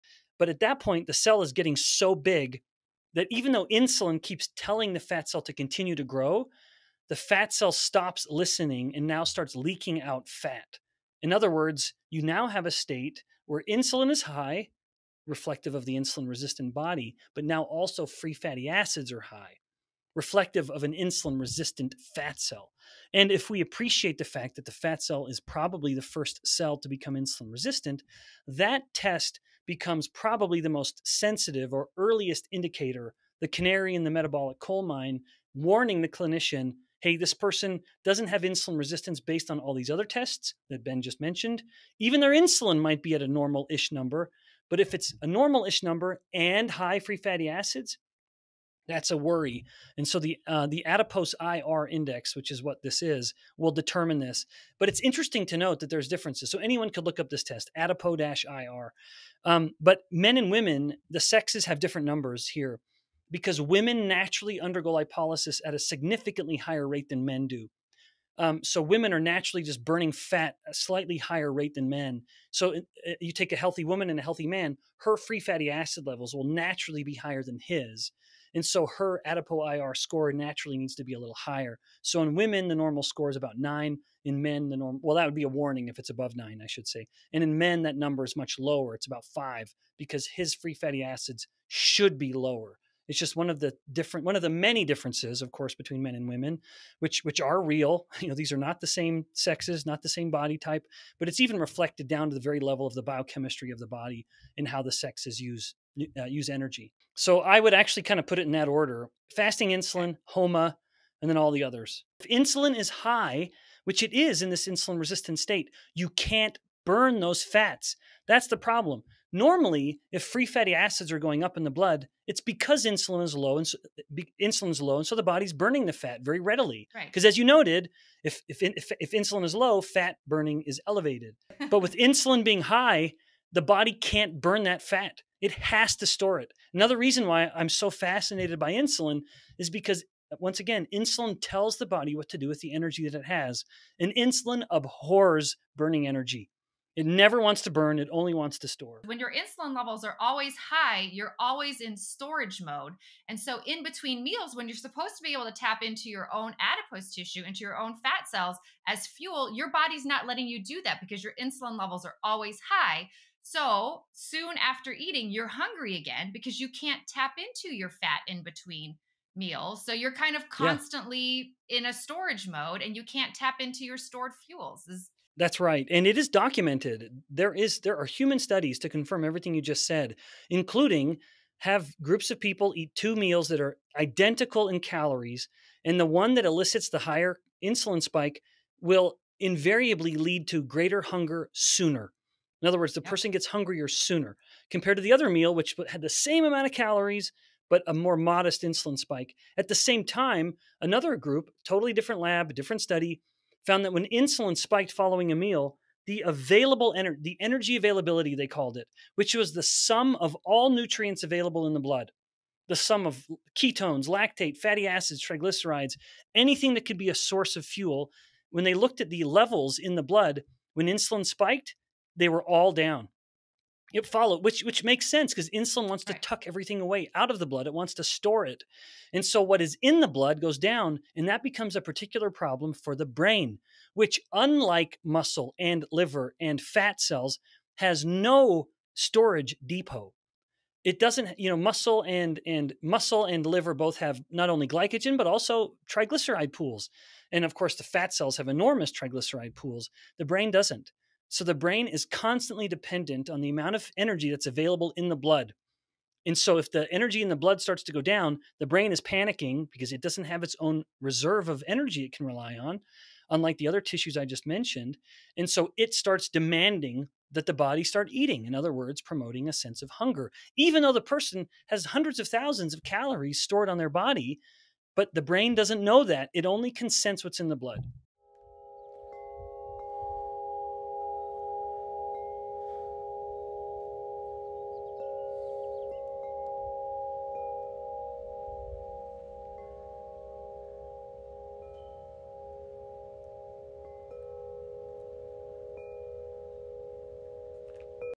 BEN BIKMAN 3~ | INSULIN HIGH? Brain demands fuel!! Drives HUNGER
presents episode 1561 | Dr BEN BIKMAN

Fat cell is so big, even though insulin tells it to grow, but
fat cell STOPS LISTENING & LEAKS FAT!
Your state now is: Insulin HIGH & Free Fatty Acids HIGH
---that is an Insulin Resistant Fat Cell.  Period.
Since fat cell is FIRST to become IR, the Free Fatty Acid
test is EARLY indicator of pre-IR!
-even fasting insulin may be close to normal
 but, if so, AND HIGH FATTY ACIDS: pre-IR
called: Adipose-IR Index

Men and women have different numbers for Adipose-IR…
-women undergo lipolysis at faster rate than men
-if both healthy, woman's adipose-IR will be higher
Women: if greater than 9: warning.  Men: if greater than 5: warning
Order of tests for IR:
-fasting insulin
-HOMA-IR
-all others

If insulin is high, as in IR, you CANNOT BURN those
FREE FATTY ACIDS….
When insulin is low [normally] then high fatty acids
in the blood are readily BURNED
Insulin tells body WHAT TO DO WITH ENERGY
Insulin Abhors Burning Energy!!

Karli Burridge: I tell my patients: when insulin high, then
between meals you are hungry due to insulin saying:
do not burn body fat...so cannot tap into stored fuel!

Back to Ben:  Human studies: 2 groups, eat same calories
-meal that spikes insulin more: group HUNGRY SOONER
Another Human Study: Energy Availability…
-sum of all nutrients available in blood
-lactate, ketones, fatty acids, TG
-when insulin spiked UP: fuels all DOWN
 -because insulin wants to store all fuels

PROBLEM for BRAIN
-unlike muscle, liver, fat cells has NO STORAGE DEPOT
 [muscle & liver both have TG & glycogen pools;
  fat cells have enormous TG pools]
Brain is constantly dependent on energy in blood
So, when insulin UP, fuel in blood DOWN;
brain panics and drive HUNGER!!!
-all the while body has tons of energy
 stored as body fat